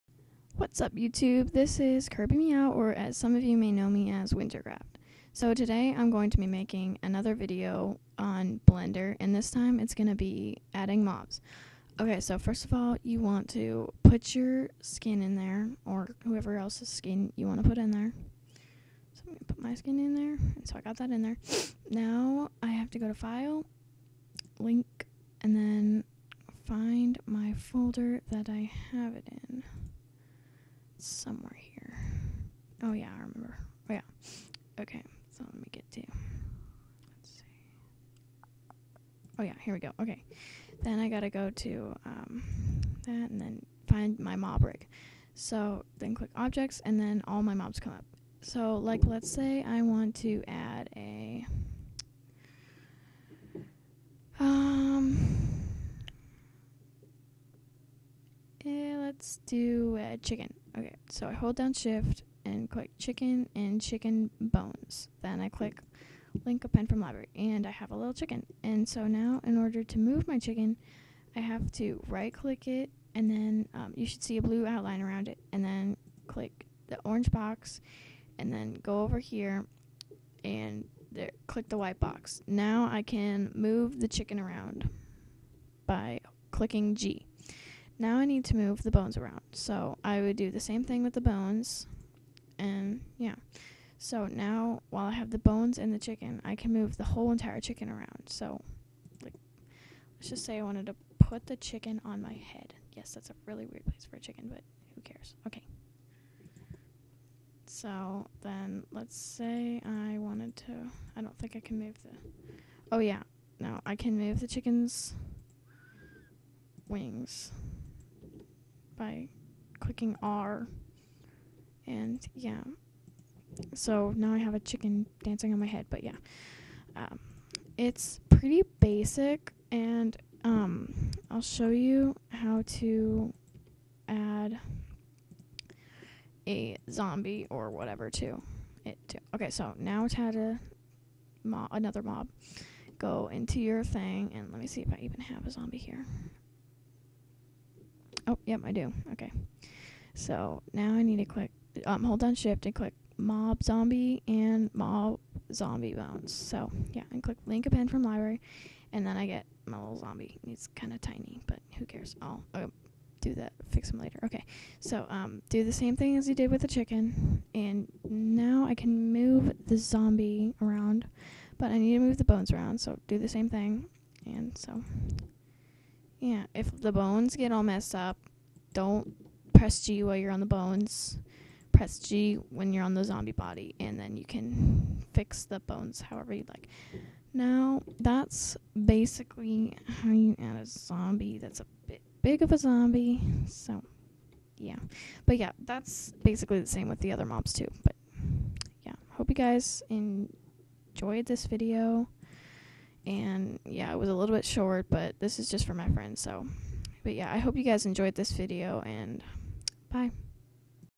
Blender tutorial~ Adding minecraft mobs
This is a Blender tutorial of how to add minecraft mobs to your render! Hope you guys enjoyed it!